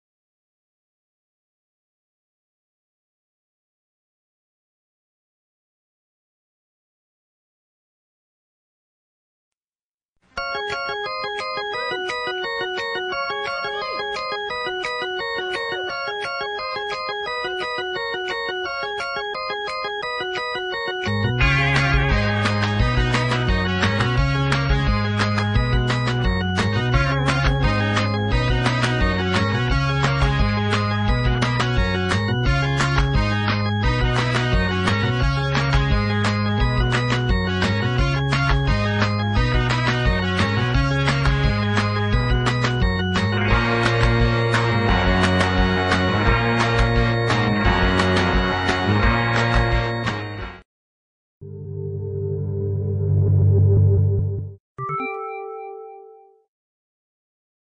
star wars animals
hi, theese are animals dressed up as star wars characters